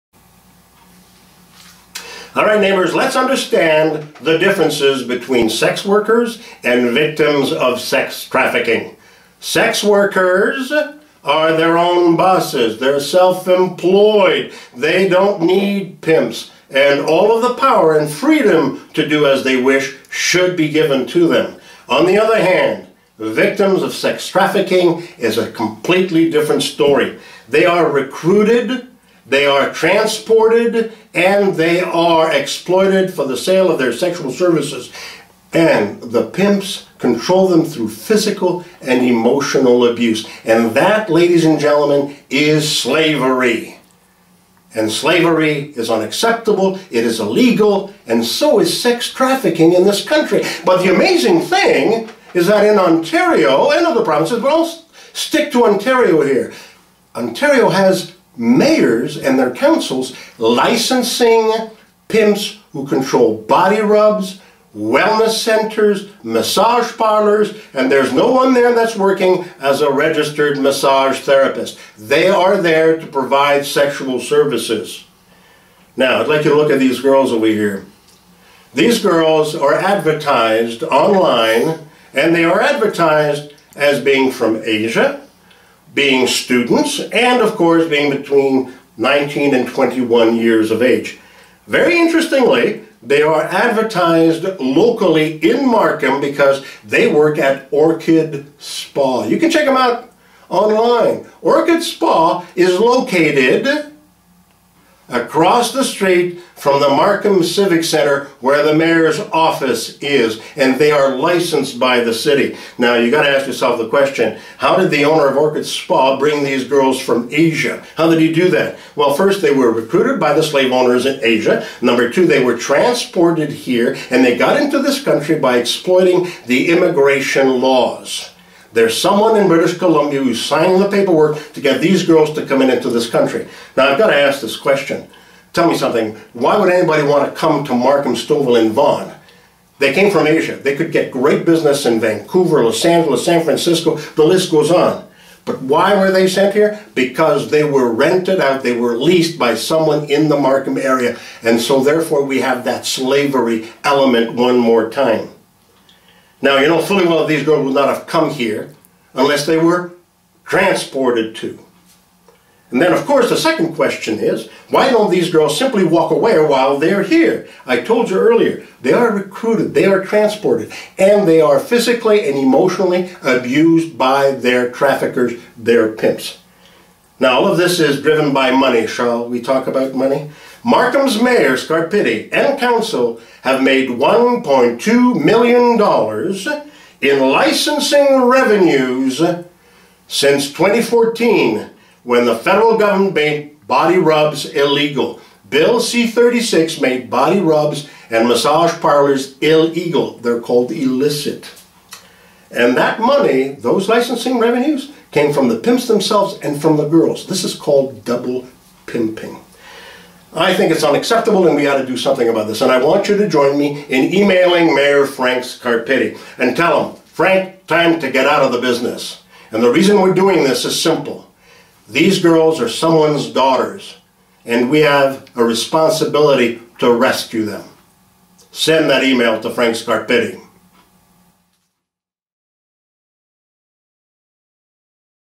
Sex Workers versus Victims of Sex Trafficking: The Difference!
In Canada, sex work is legal. Sex trafficking, on the other hand, is a crime.